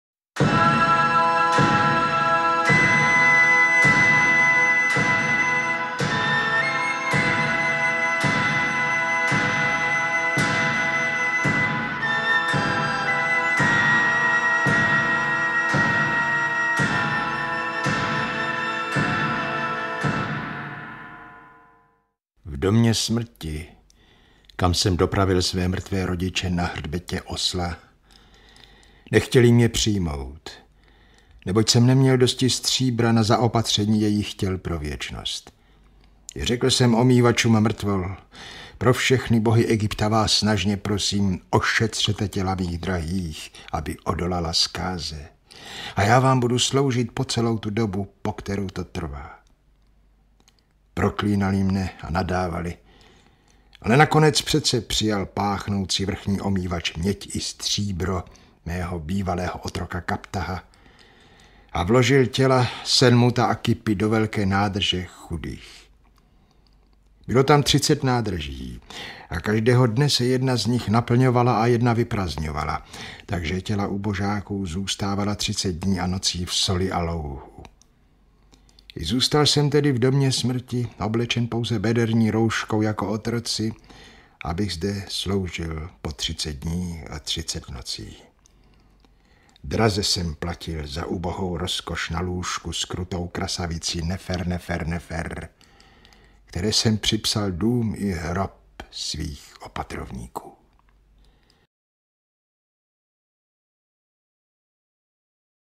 CD mp3 Mika Waltari - Egypťan Sinuhet - ukázka
Vydal Radioservis, a.s., 2011

čte Josef Červinka, rozhlasová nahrávka roku 1983

Četba z jednoho z nejvýznamnějších světových románů 20. století. Mika Waltari (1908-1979) dopsal a dal k vydání román Egypťan Sinuhet v roce 1945 a ačkoli popisoval život egyptského lékaře doby největších faraonů, vypovídal především o válečných traumatech jeho současníků. Sinuhet se bez většího přičinění dostává k nejvyšším vládců své doby a ovlivňuje osudy celého státu, aniž může ovlivnit život svůj a svých nejbližších. Jeho život není šťastný, protože okamžiky štěstí si neuvědomuje dříve, než zasáhne temný osud.